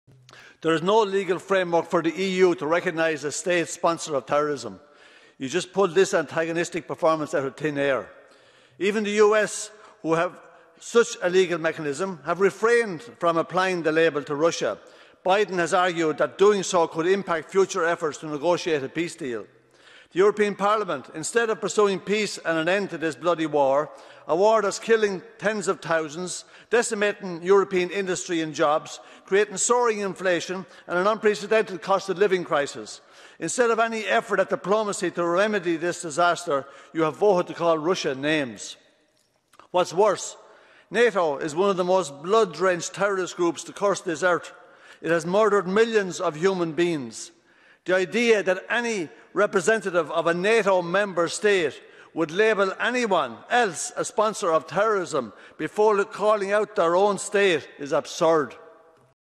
Non esiste un quadro giuridico per l'UE per riconoscere uno Stato sponsor del terrorismo. 🇮🇪
Mick Wallace, Eurodeputato indipendente. 🇮🇪

Anche gli USA si sono rifiutati di applicare l'etichetta alla Russia. L'idea che chiunque provenga da uno Stato NATO inzuppato di sangue etichetti qualcun altro come sponsor del terrorismo prima di chiamare in causa il proprio di Stato, è assurda.